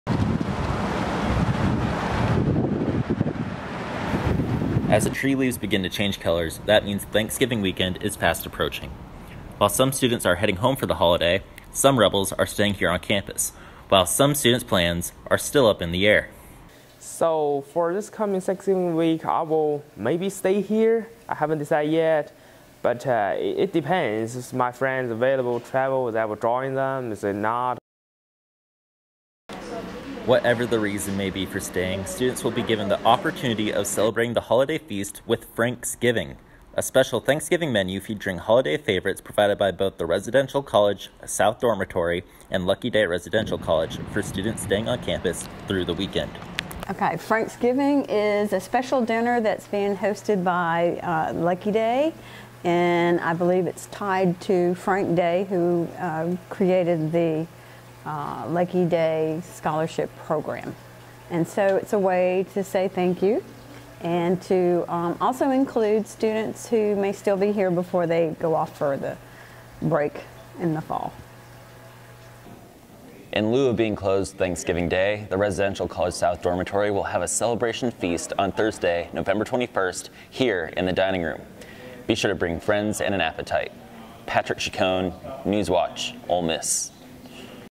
FRANKSGIVING -- THANKSGIVING AT OLE MISS
With Thanksgiving Weekend fast approaching the dormitories on campus are providing a Thanksgiving Feast for those staying on campus through the holiday. Patrick Chacone has the story.